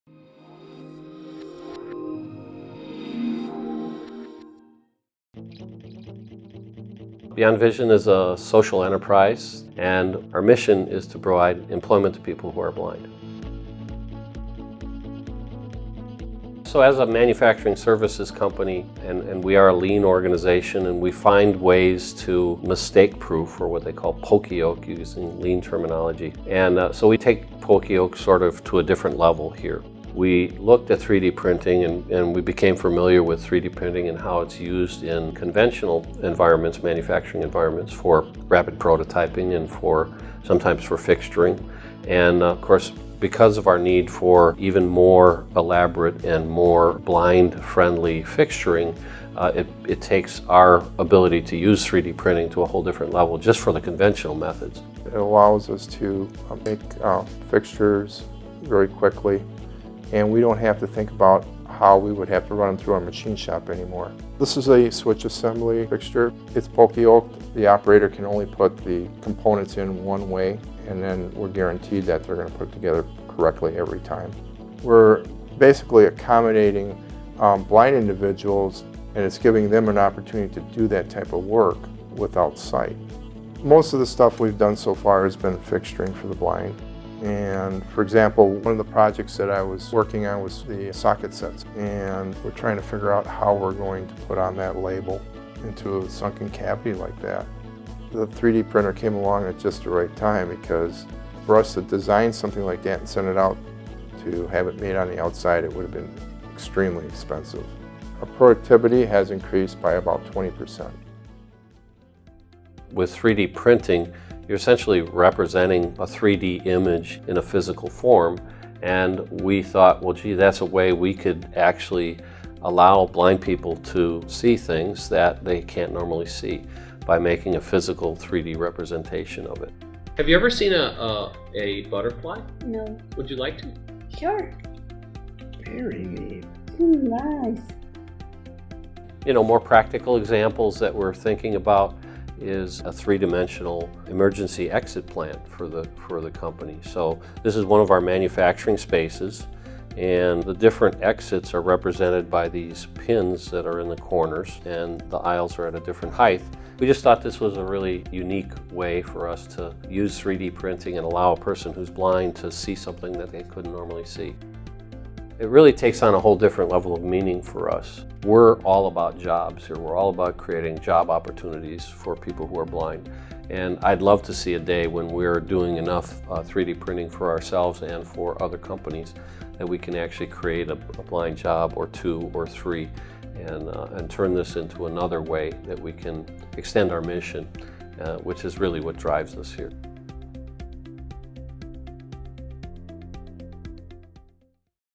Beyond Vision - 3D Printing Success Story
By utilizing in-house 3D printing, Beyond Vision is creating mistake-proof fixtures to further accommodate their visually impaired employees.